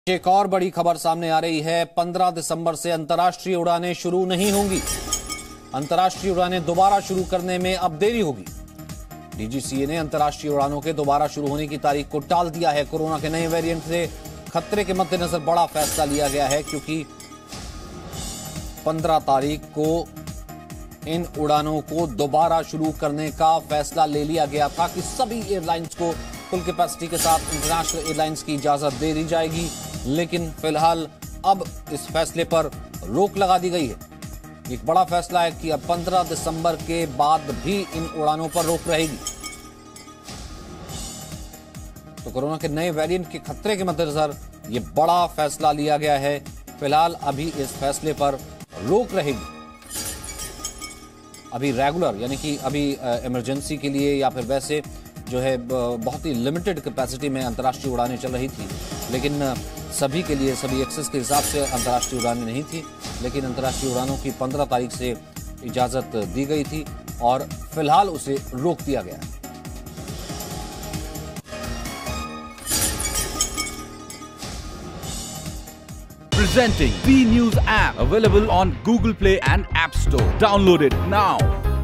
DGCA ने अंतरराष्ट्रीय उड़ानें शुरू होने की तारीख टाली | Internation Flights | Omicron |Covid | Delhi
कोरोना के नए वैरिएंट के खतरे को ध्यान में रखते हुए बड़ा फैसला लिया गया. DGCA ने अंतरराष्ट्रीय उड़ानें शुरू होने की तारीख टाली.

Keeping in mind the danger of the new variant of Corona, a big decision was taken. DGCA postponed the date of commencement of international flights.